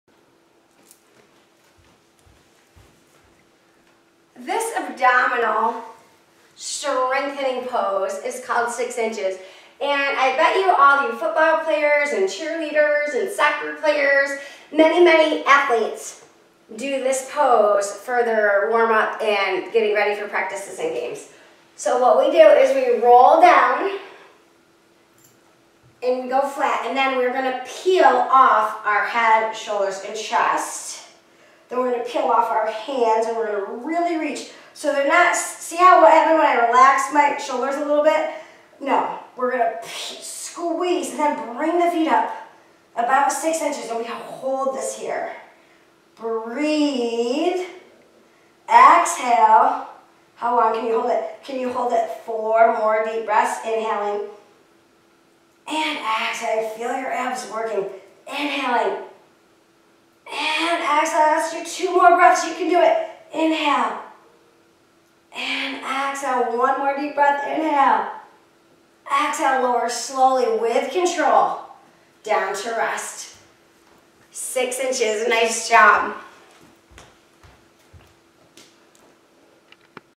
6 Inches
The longer you can hold this position, the stronger your abs will become!